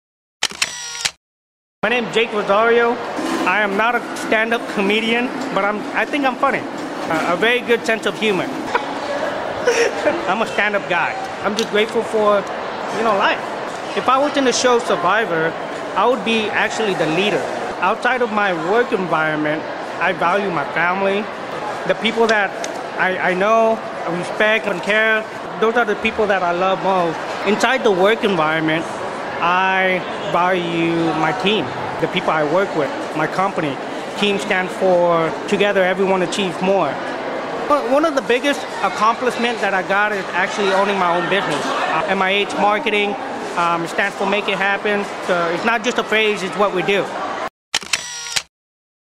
Jake Rosario - a Mini-Mentor Trailer
mcGyver Represents a trademarked WFC mini-Mentor Trailer produced by mcGyver Represents. Jake Rosario of MIH Marketing expresses Team and Making It Happen